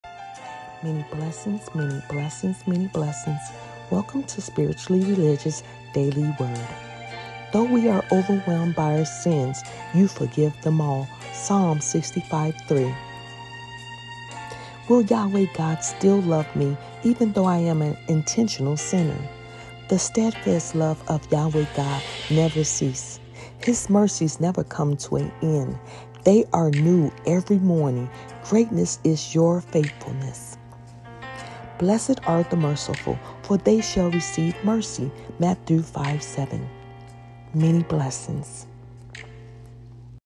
Does Yahweh love me? I am a sinner
Spiritually Religious Daily Word
Will Yahweh God forgive me even though I am an intentional Sinner?

Yahweh God is a loving forgiving God. He let us know that He is Faithful even when we are not faithful to Him. When you are doing something and you know that it is not right in Yahweh God Laws. Surrender to Yahweh God and Trust Him and pray to Him asking that you remember that you have already gave it to Him. Have that Faith of a mustard seed knowing that Yahweh God will stay faithful to you Always. Do not be anxious about anything but in every situation by prayer and petition with thanksgiving present your requests to Yahweh God.